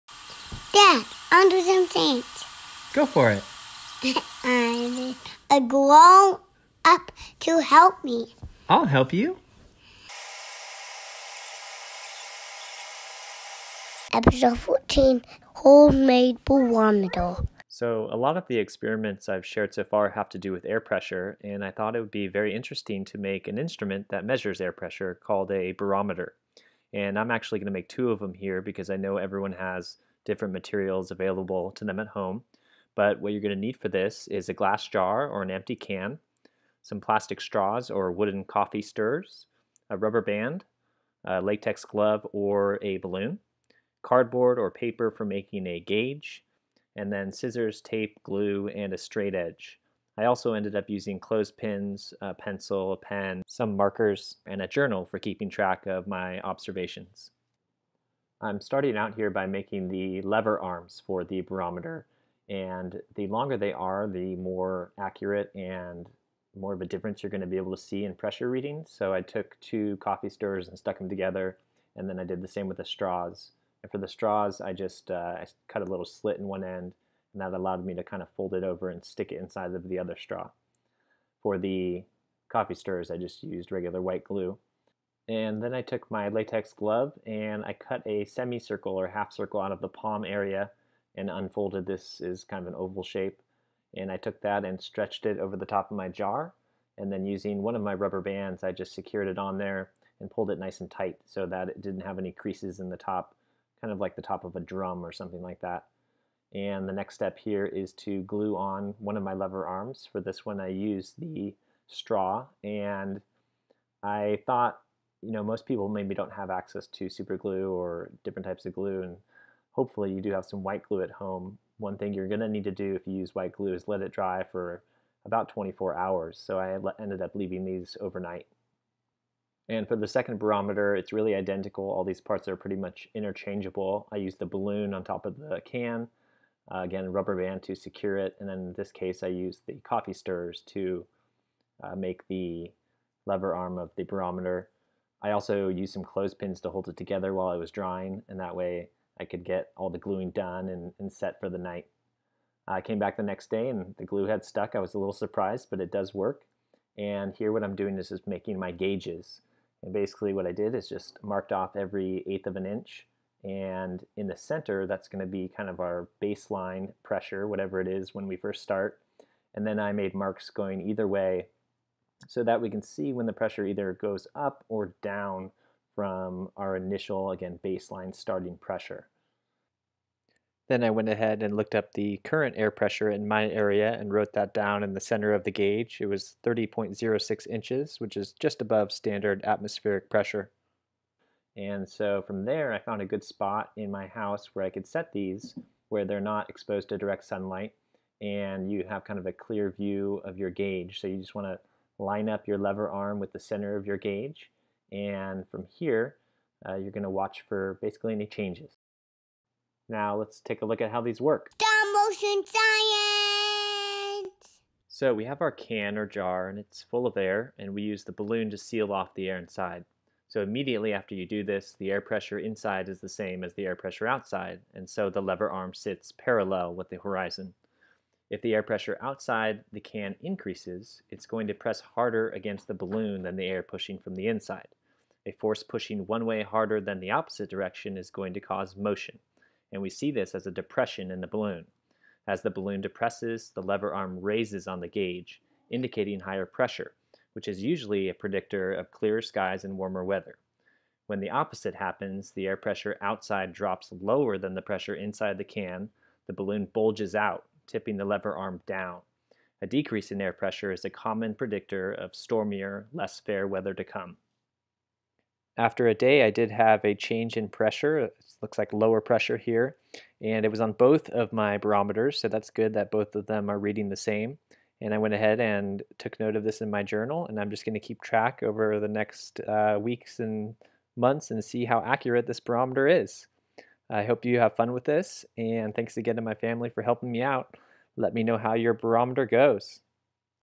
Episode 14: Homemade Barometer
All these experiments with air pressure, we need a scientific instrument to measure it!

Homemade Barometer Materials Used:
Glass Jar or Empty Steel Can
Plastic Straws or Wooden Coffee Stirrers
Rubber Band
Latex Glove or Balloon
Cardboard or Paper for Making a Gauge
Scissors
Masking Tape
White Glue
Ruler or Straight Edge
Clothespins
Pencil
Pen
Markers
Journal